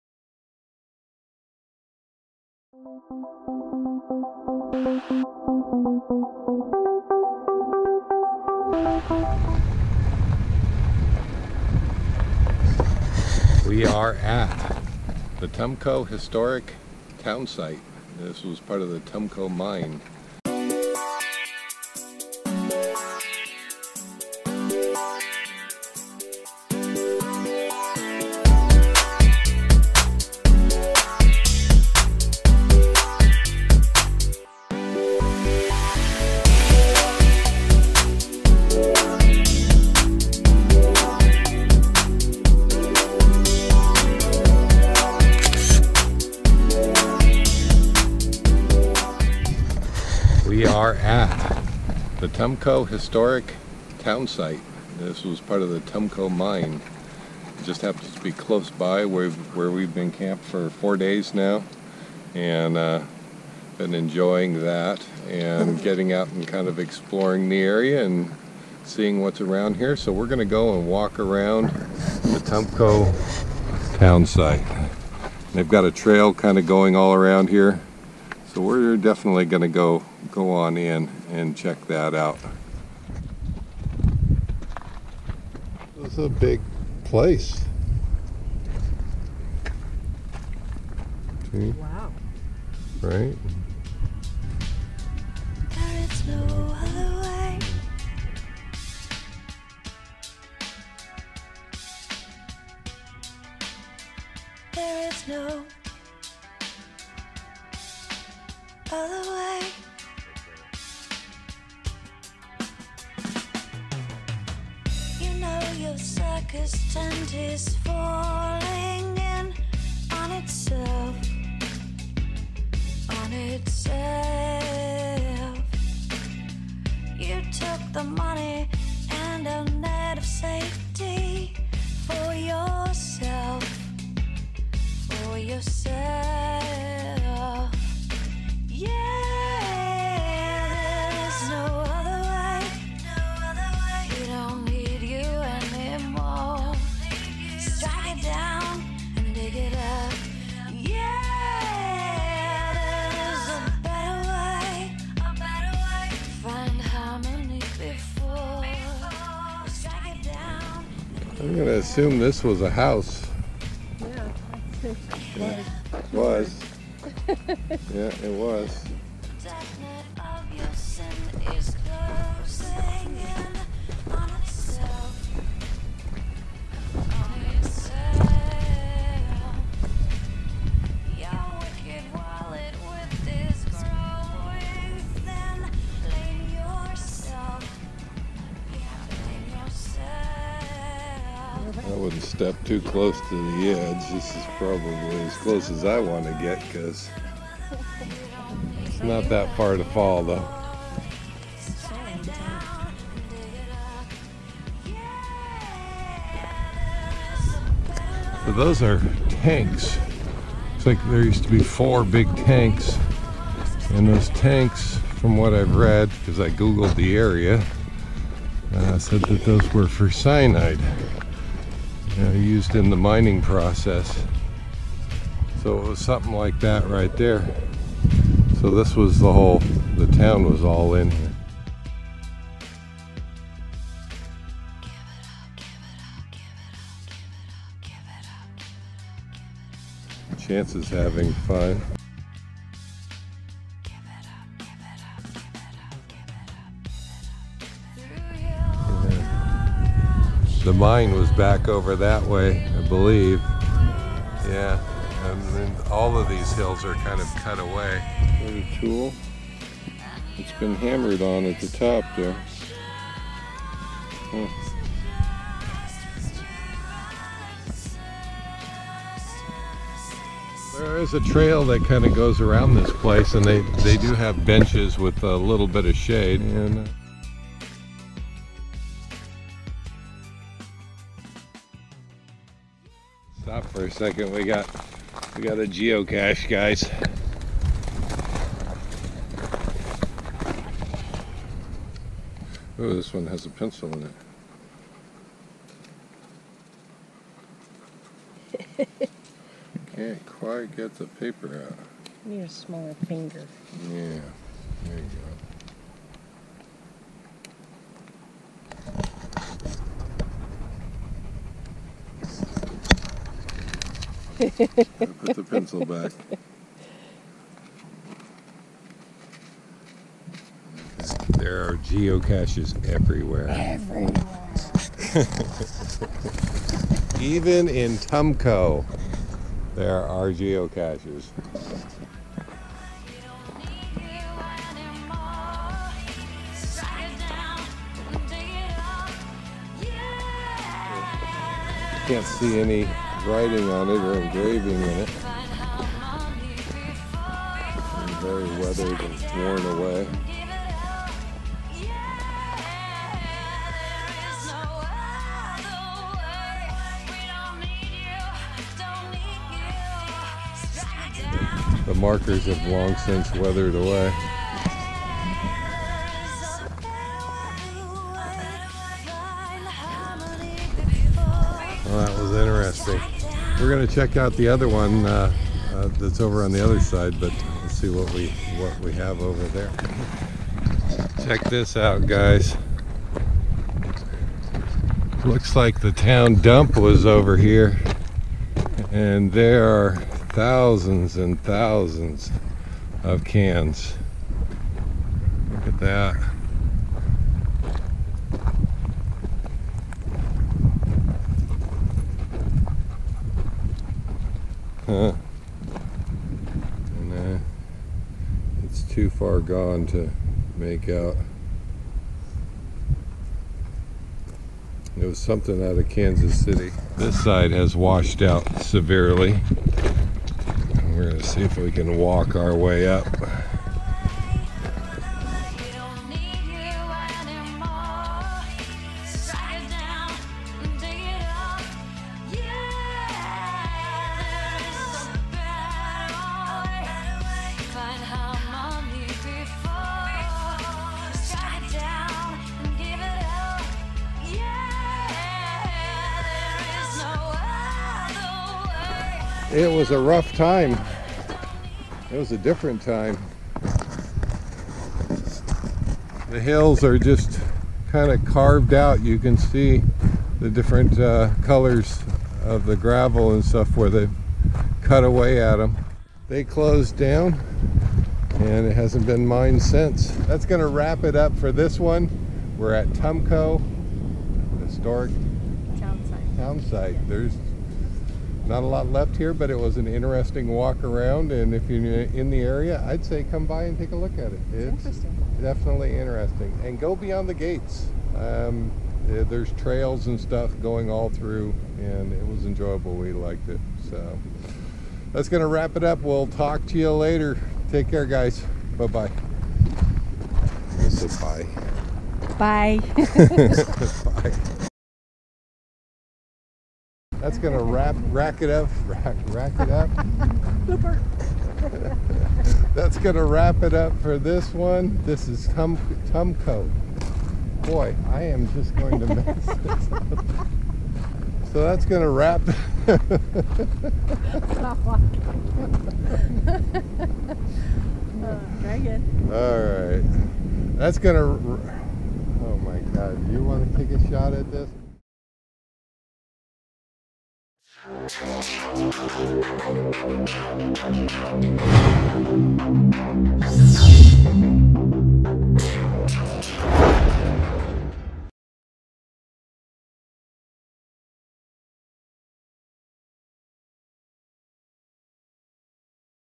Tumco California Historic Townsite
Tumco is an abandoned gold mining town and is also one of the earliest gold mining areas in California. It has a history spanning some 300 years, with several periods of boom and bust. Originally named Hedges, the town was completely abandoned in 1905 although some mining still went on until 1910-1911.  We really enjoyed this adventure and we hope you enjoy the video.

Cheers,
Karen & Dave